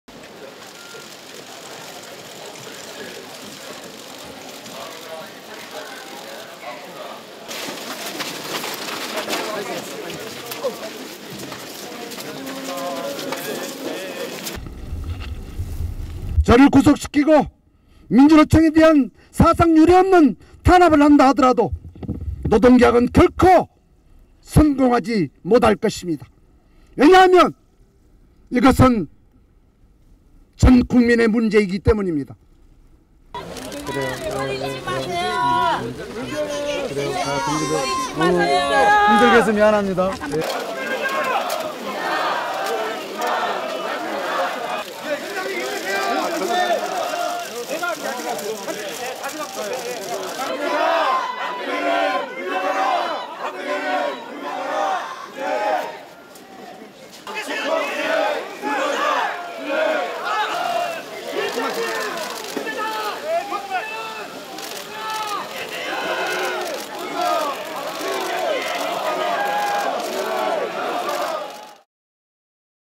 뉴스타파 - 한상균 위원장 "노동개악 성공할 수 없을 것" (2015.12.10)
한상균 민주노총 위원장이 10일 경찰에 자진 출두했습니다. 올해 세월호 희생자 추모집회와 노동자대회 등을 주최해 도로교통법과 집시법 위반 혐의로 체포 영장이 발부돼 조계사에 은신한 지 25일 만입니다.